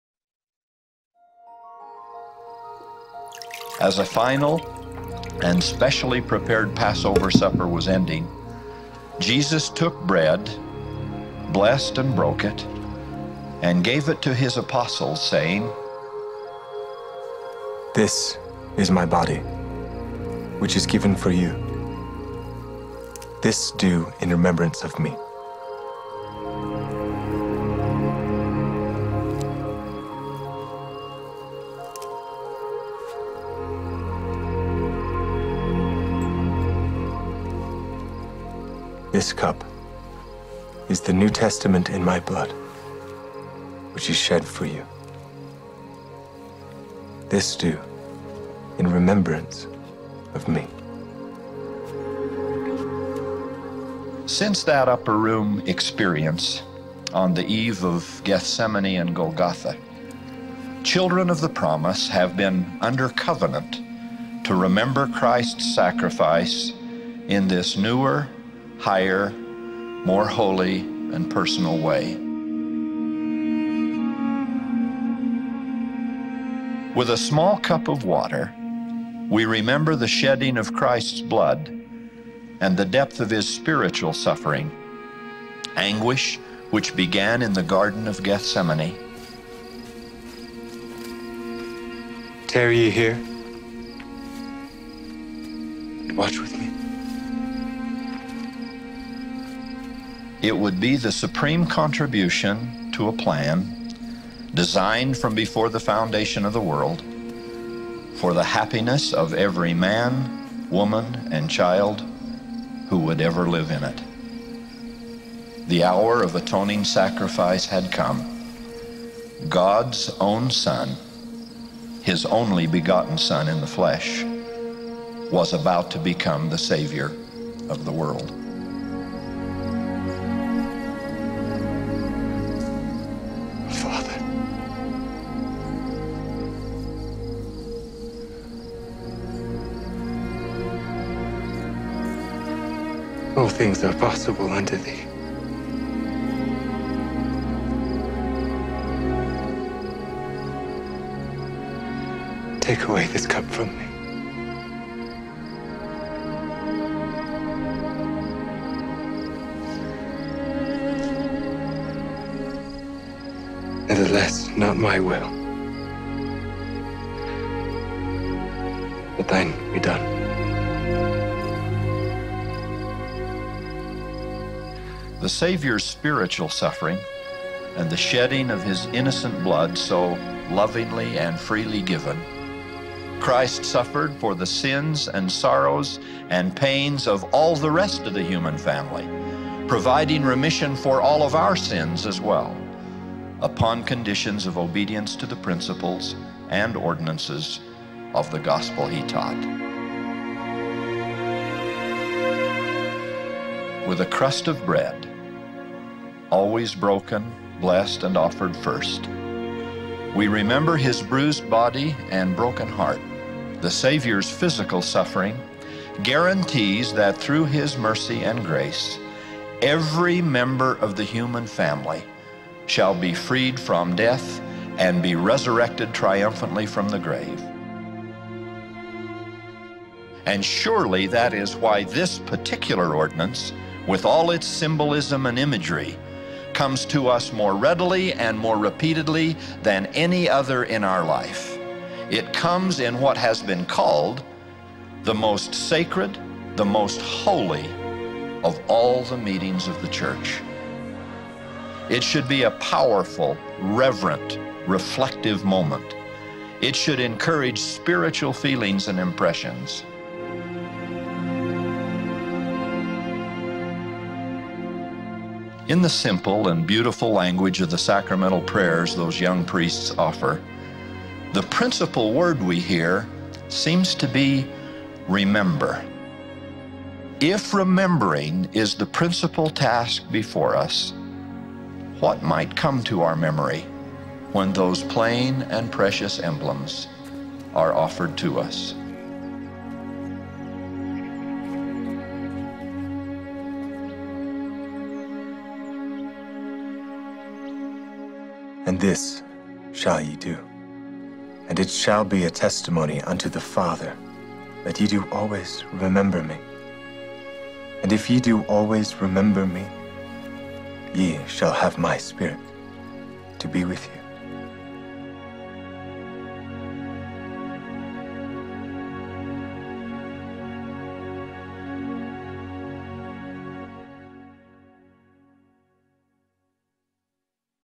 Always Remember Him
The sacrament is an ordinance in which Church members partake of bread and water in remembrance of the sacrifice of Jesus Christ. Through this ordinance, Church members renew the covenants they made with God when they were baptized.